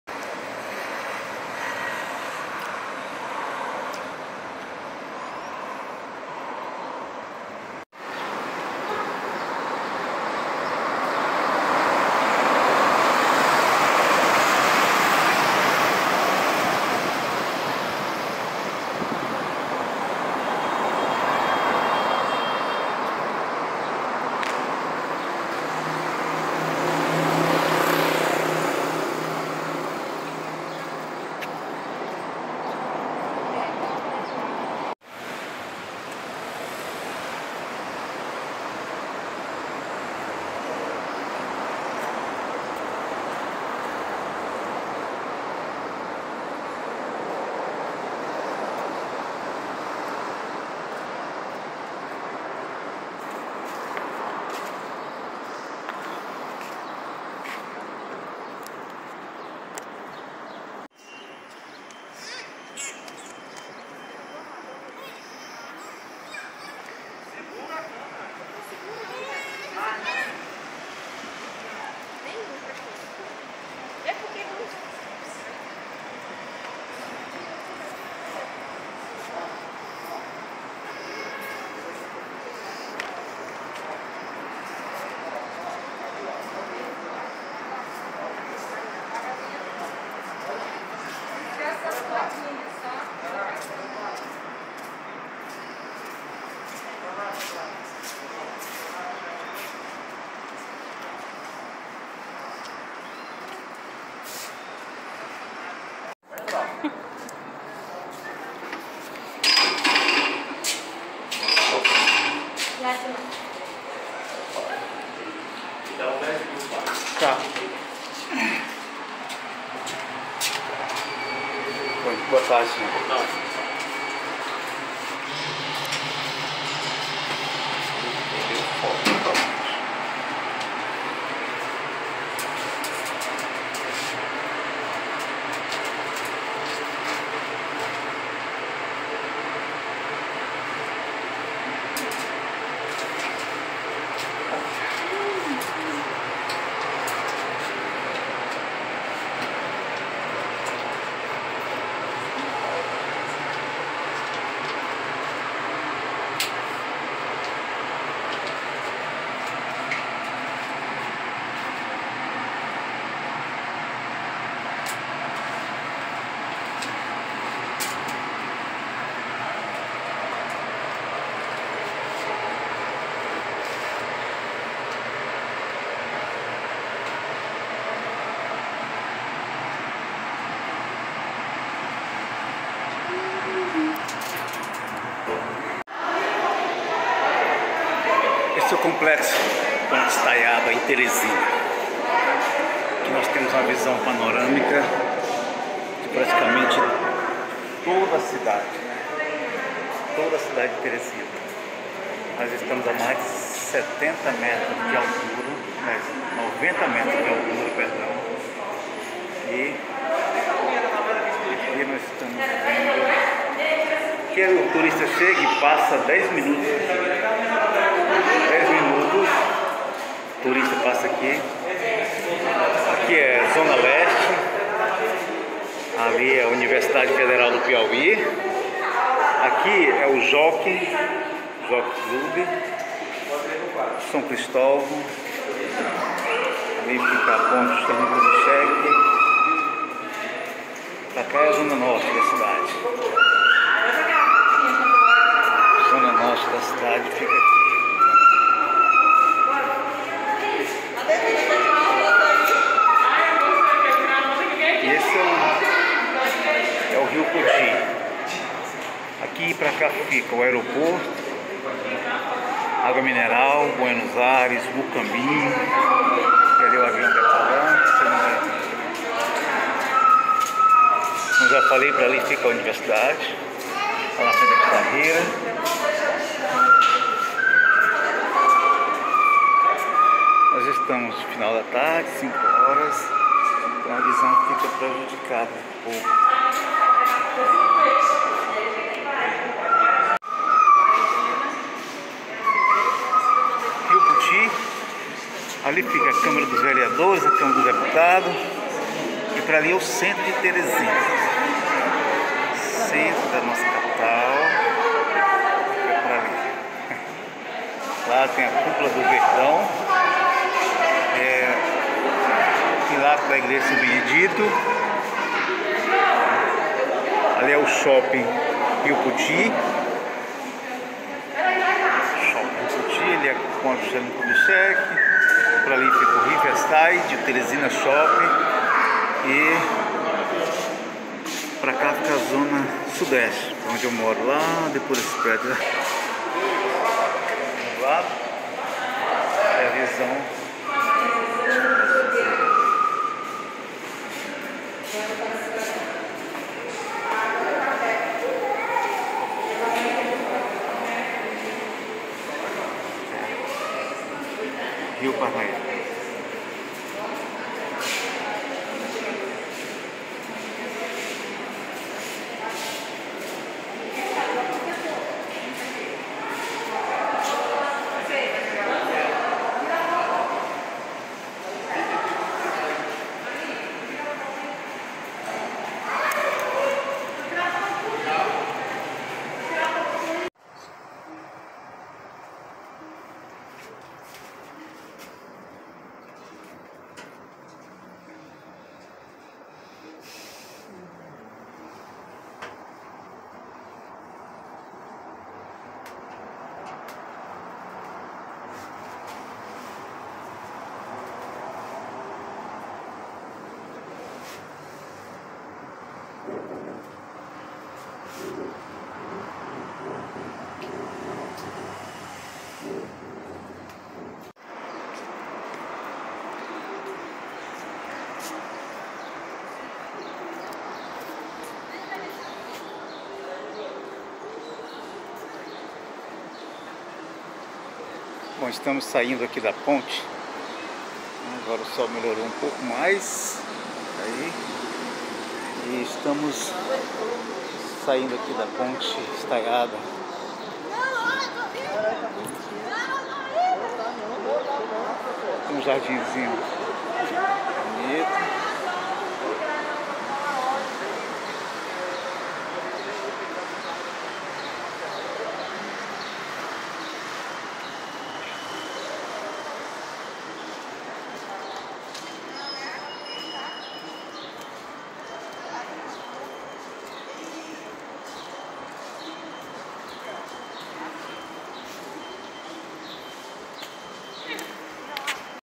Teresina. Mirante Ponte Estaiada
uma linda visão na ponte mete João ISidoro França em Teresina norderte do Brasil